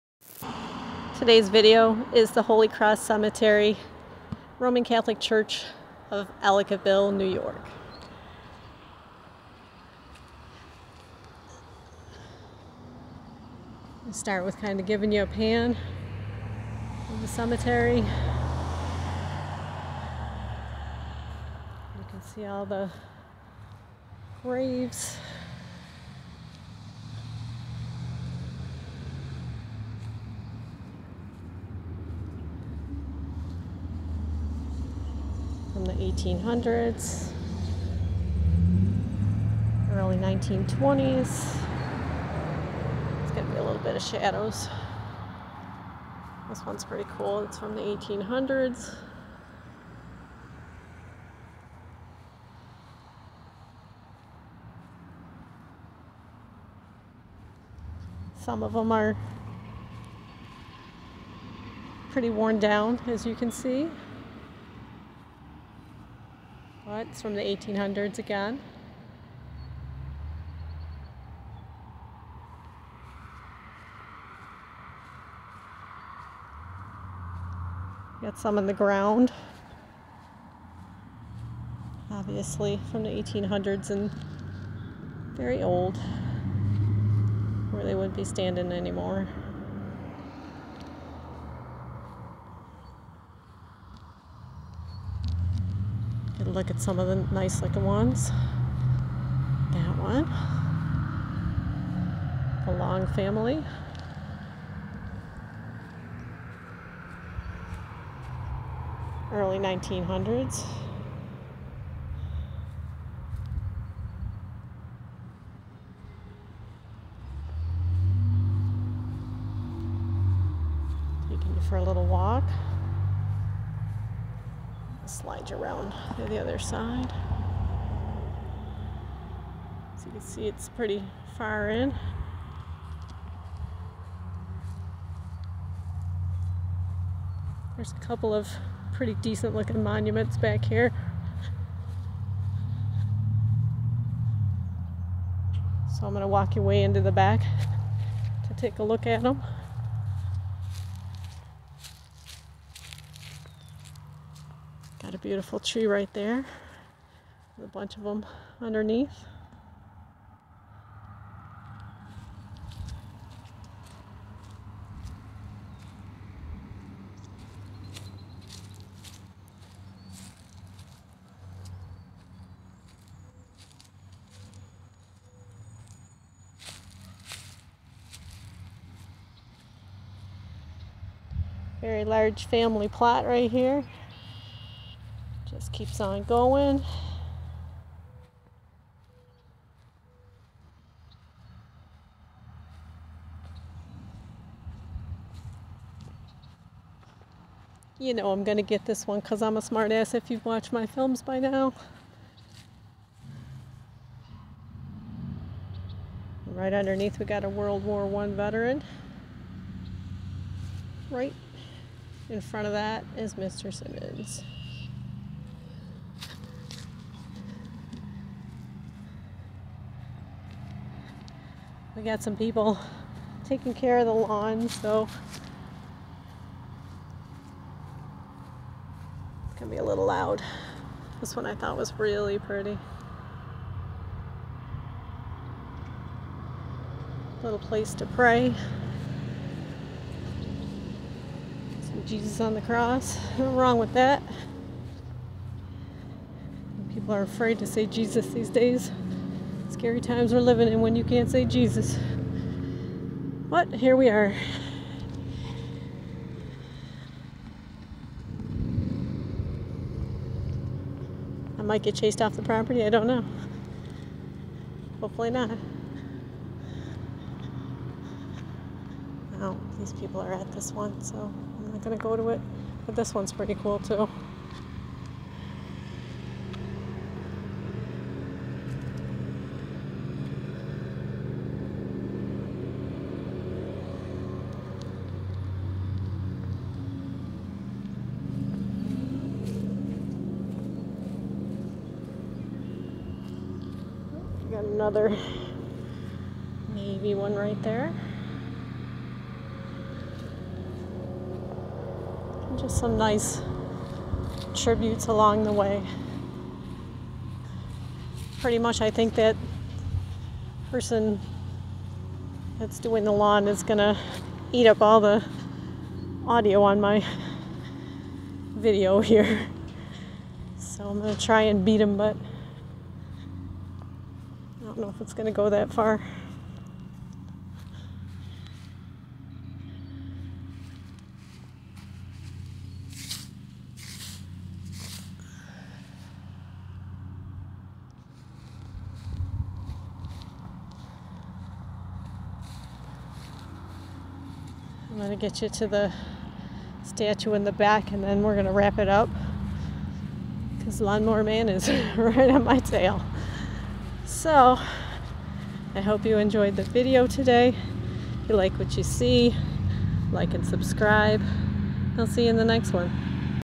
Holy Cross Cemetery- Ellicottville, NY
Located at the junction of the 219 & 242, this cemetery is easily viewed from anyone coming into Ellicottville (affectionately named EVL by the locals).  It has many military Veterans and a rich history inside.